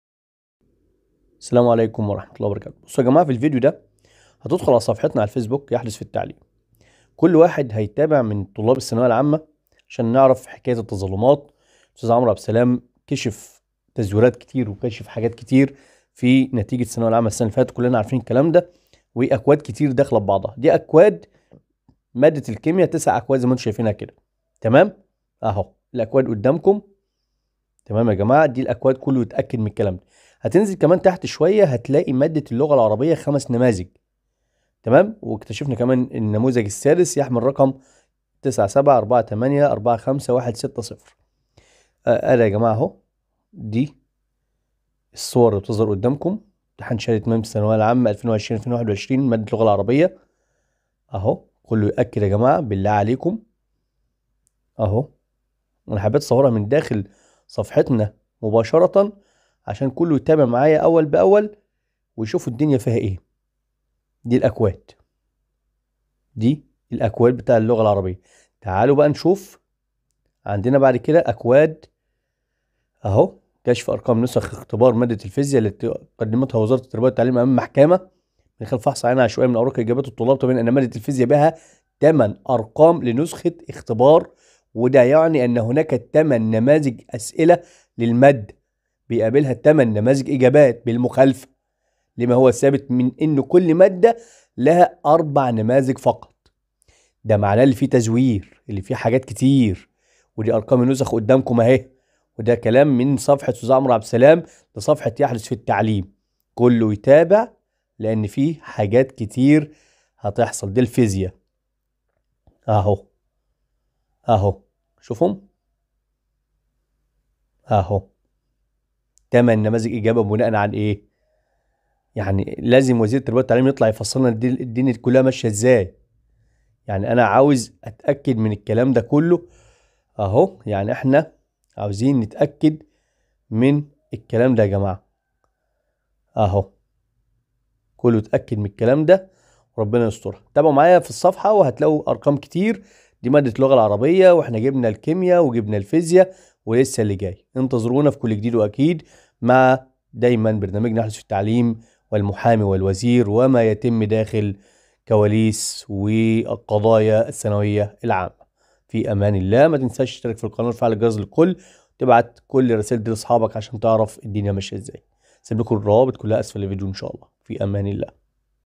عاجل الان كشف تزوير اوراق إجابات الثانوية العامة علي يد محامي الطلبه بالمستندات.. من المسؤوووول ؟!🤔🧐
بسم الله  نبدأ كشف  أرقام  نسخ اختبار  مادة الفيزياء  التي قدمتها  وزاره التربية والتعليم أمام  محاكم القضاء الاداري علي مستوي  الجمهورية
من خلال فحص  عينة عشوائية  من أوراق إجابات الطلاب  تبين  أن  مادة الفيزياء  يوجد لها ٨ أرقام لنسخة الاختبار  وهو ما يعني  أن هناك  ٨نماذج أسئلة للمادة يقابلها ٨ نماذج إجابة
بالمخالفة لما  هو  ثابت من أن كل مادة يوجد لها ٤ نماذج ففط

أرقام نسخ الاختبار  كماهو موضح بالصور كالآتي :_
١_        ٨٨٣١٠٠٣٦٠

٢_      ٤٧٣٦٧٩٠٥٢

٣_    ٦٨٨٢٢٧٧٢٤

٤_    ٦٥٧٥٤١٢٠٣

٥_    ١٠٥٤٨٦٣٣٩

٦_   ١٧٣٠٧٠٤٢٤

٧_  ٦٩٩٣٨٩٣٧٢

٨_  ٦٠٢٢٠٦٦٠٨
عمرو عبدالسلام المحامي بالنقض
 يتبع باقي المواد
النموذج السادس للغة العربية يحمل رقم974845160
بخلاف النموذج إللي بيحمل رقم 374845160
مادة اللغة العربية  ٥ نماذج
٢٠٤٧٦٦٧٣٥

٤٠٨٣٦٤٧٤٩

٨٧٥١٢٢٠٦٧

٣٧٤٨٤٥١٦٠

٤٩٣٦٦٠٢١٦
اكتشفنا النموذج السادس يحمل رقم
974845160
ارقام اكواد مادة الكيمياء ٩اكواد
ارقام الاكواد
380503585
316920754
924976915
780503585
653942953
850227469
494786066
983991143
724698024

☎️للاتصال بنا من هنا 👍👇👇👇👇👇
👈❣للإستفسار والاعلانات 👍
اجيال الاندلس مدرستنا | ابتدائي. اعدادي. ثانوي